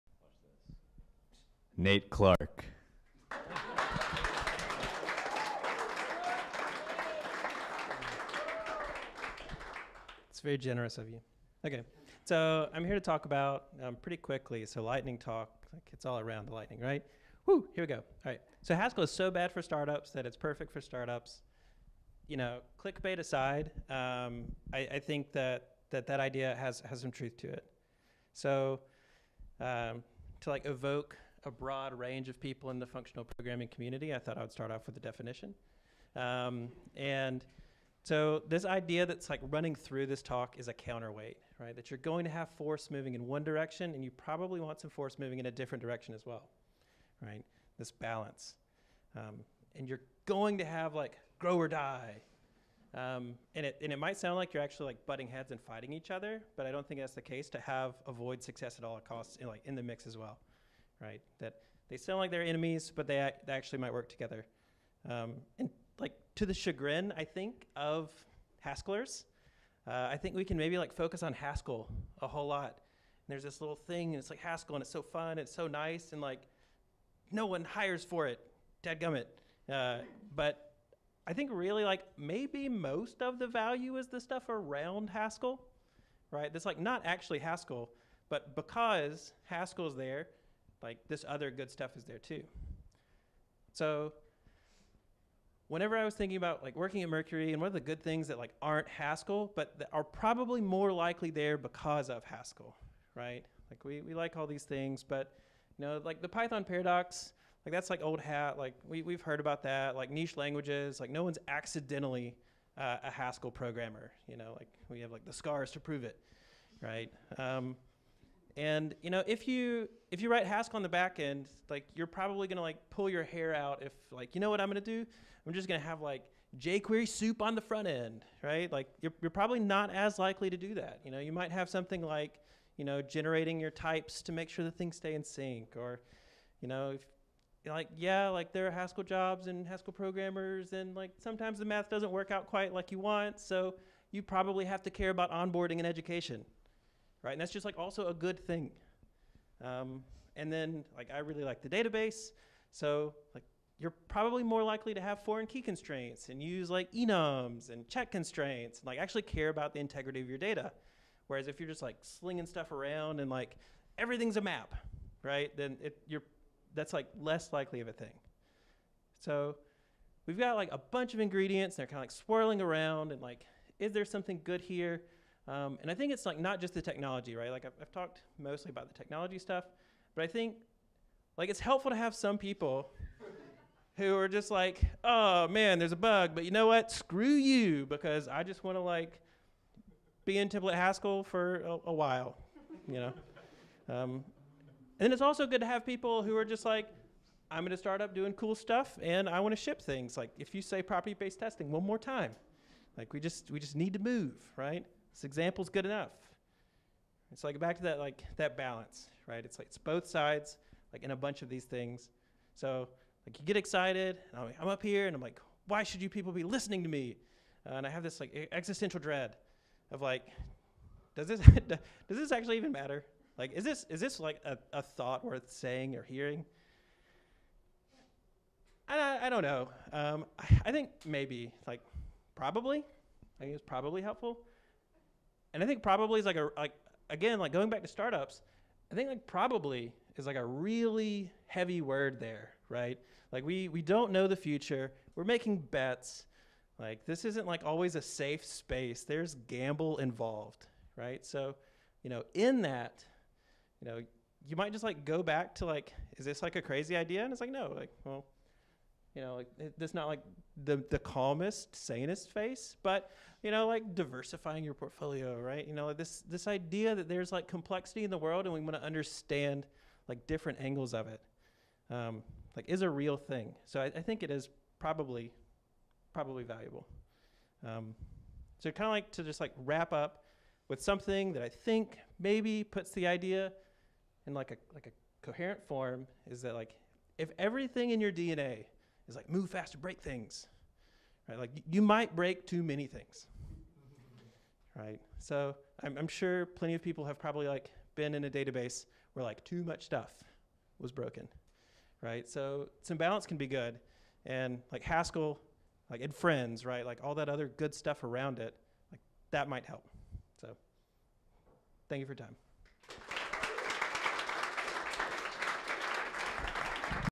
Haskell talks at Mercury - Haskell is so bad for startups that it's perfect for startups, Nate Clark
Haskell is so bad for startups that it's perfect for startups, by Nate Clark